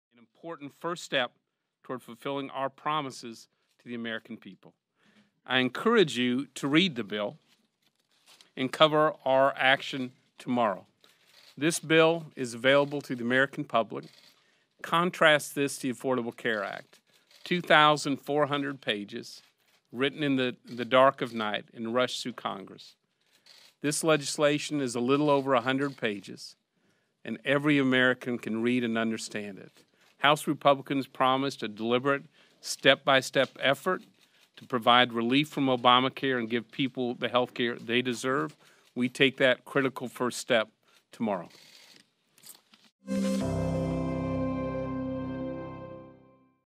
Republicans Complain About How Fast Obamacare Passed
One of the authors of the GOP's health care replacement bill complained about how quickly Obamacare passed in 2010, despite the fact that Republicans are trying to push their legislation through in less time.